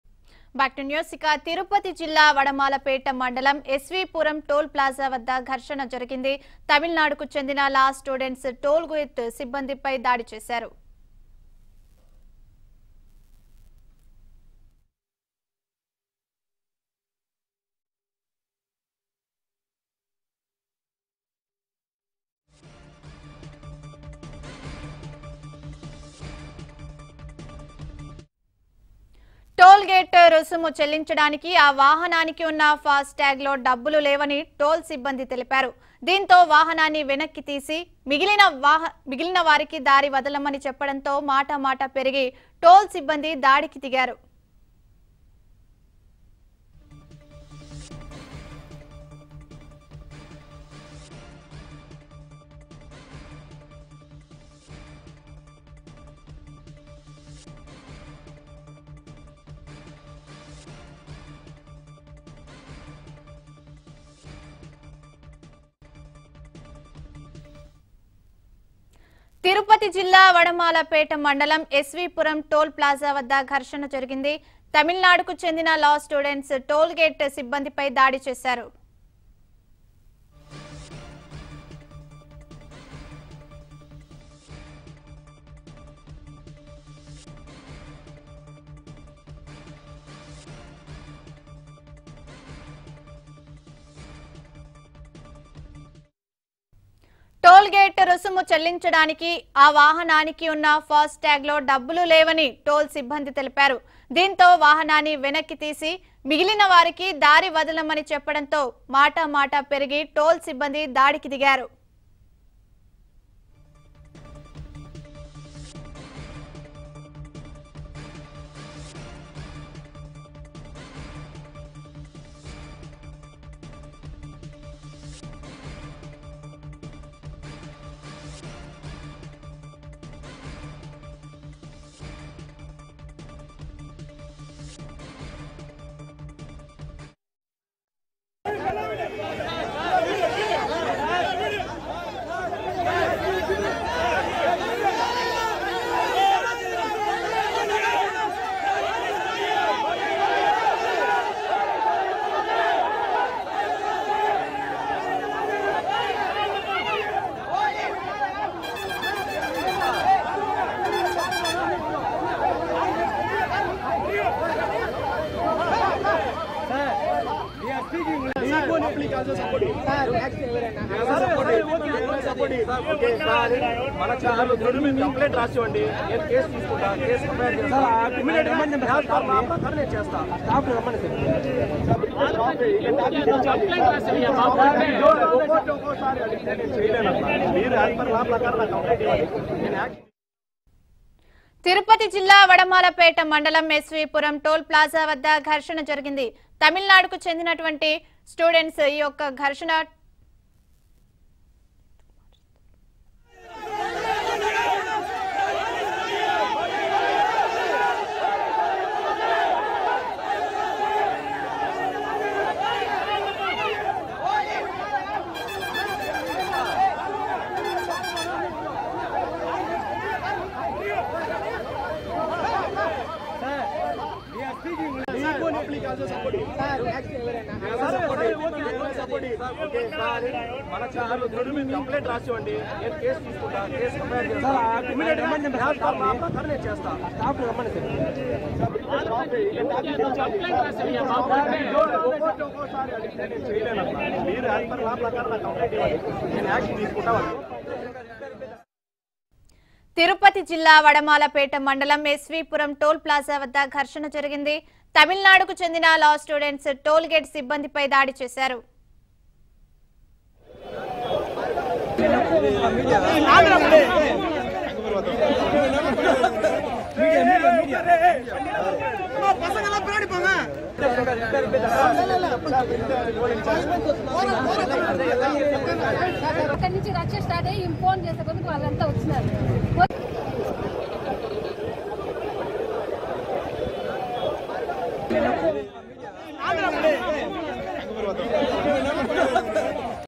ఎస్వీపురం టోల్ గేట్ సిబ్బంది పై తమిళనాడు లా స్టూడెంట్స్ దాడి.. || Tirupati | ABN Telugu
ఎస్వీపురం టోల్ గేట్ సిబ్బంది పై తమిళనాడు లా స్టూడెంట్స్ దాడి.. || Tirupathi | ABN Telugu